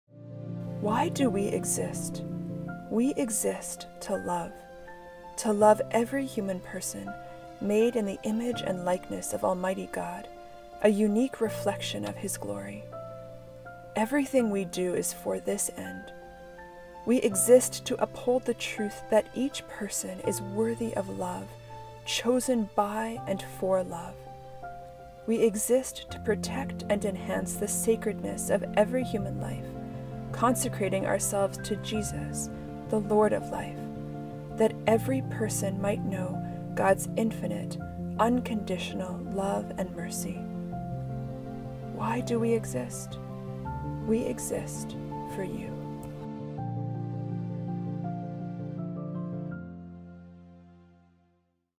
Sisters of Life - Why We Exist
A brief look at the Sisters of Life, and why we exist.